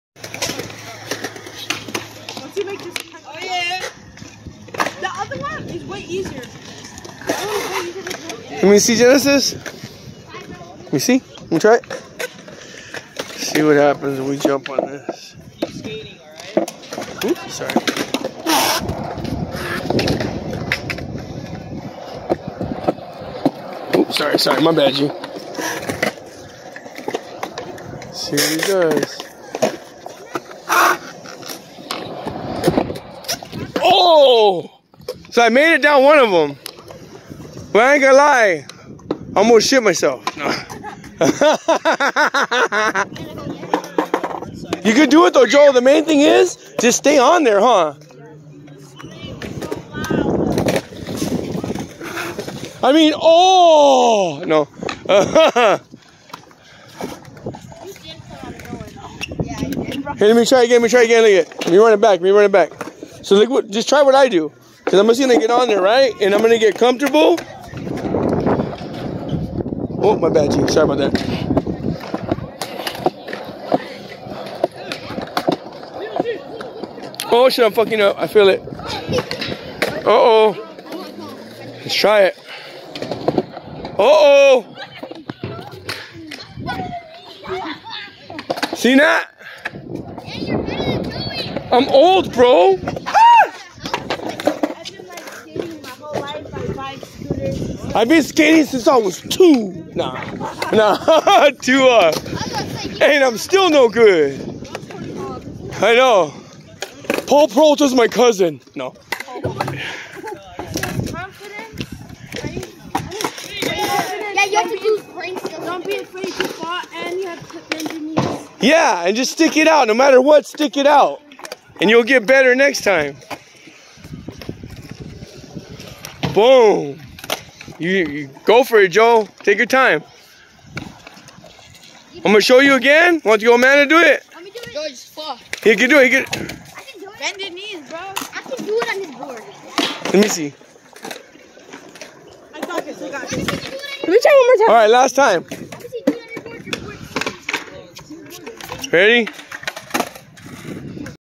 JoJo's 12 years old and he says now it's time to skate at the skatepark he wants to Master skating
if you work hard at any goal you have you can achieve it